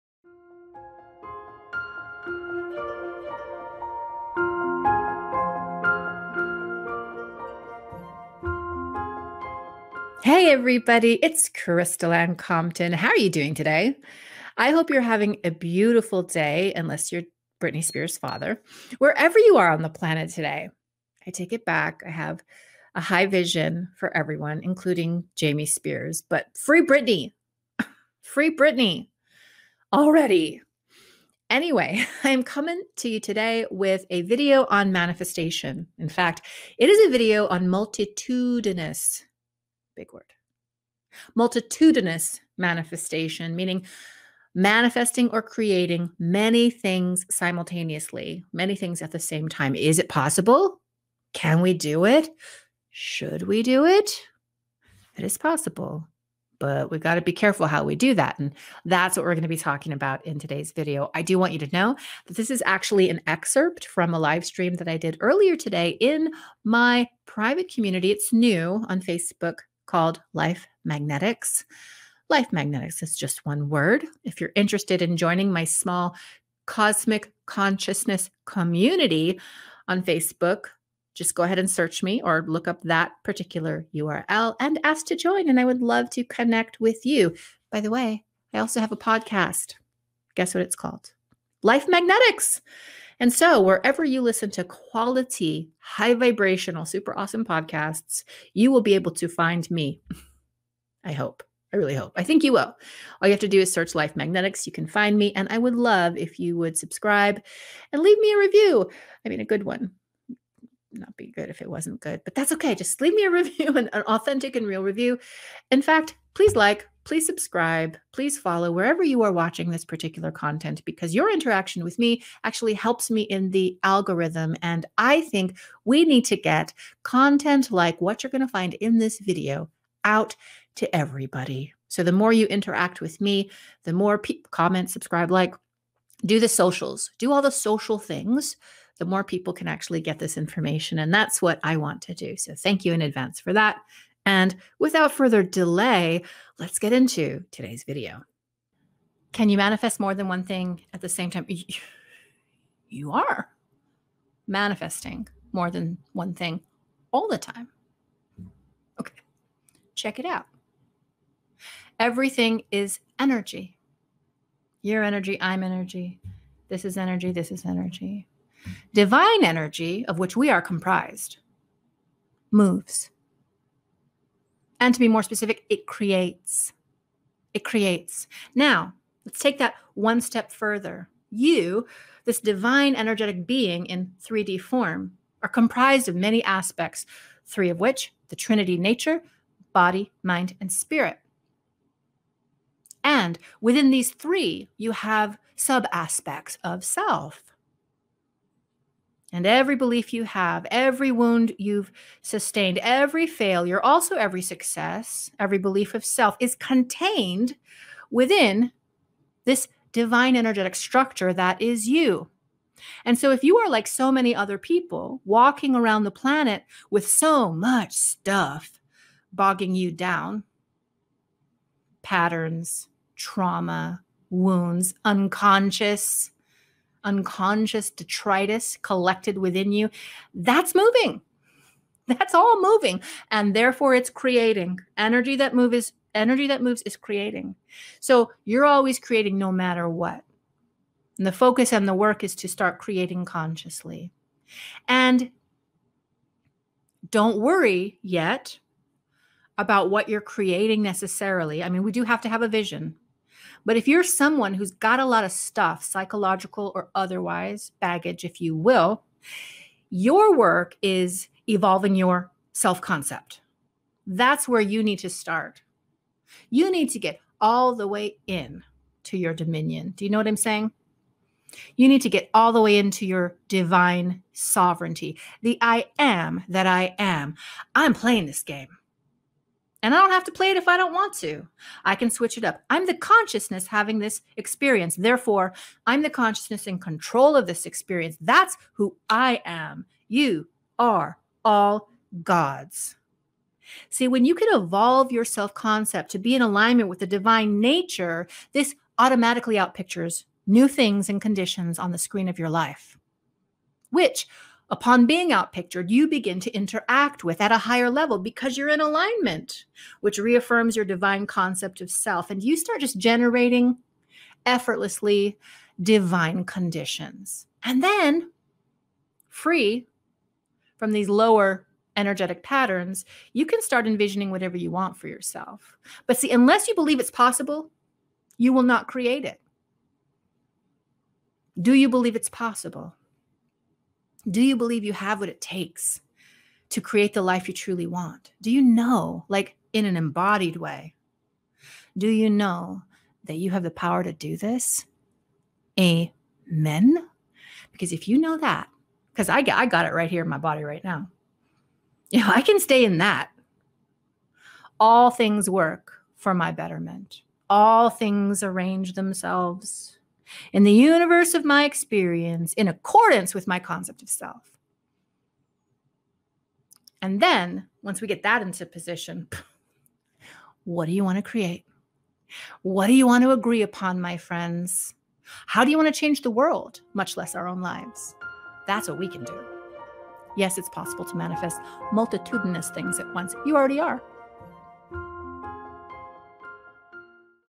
The Reason You Can't Manifest (and also how you can)
You're a conscious creator, can you dig it! You're creating all the time. The question is, WHAT are you creating? Do you even know?

Find Life Magnetics Podcast wherever you listen to podcasts or go